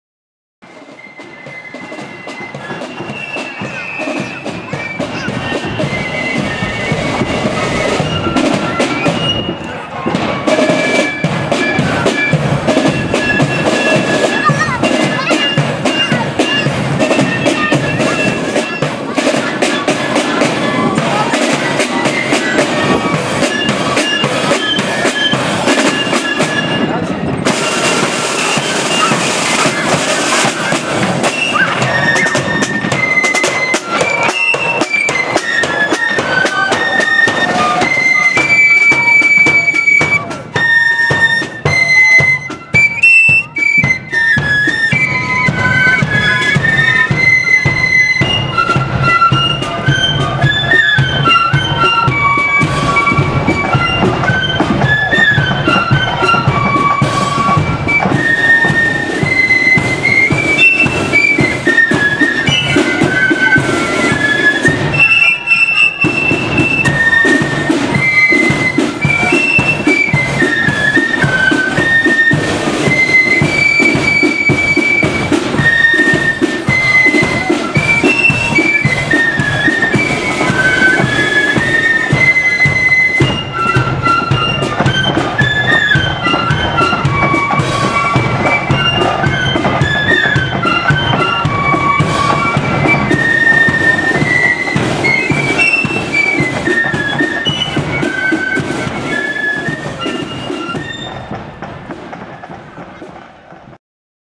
Pride of the Raven @ East Belfast Protestant Boys Parade 2016
Pride of the Raven on the march in East Belfast for the Protestant Boys annual band parade Saturday 14th May 2016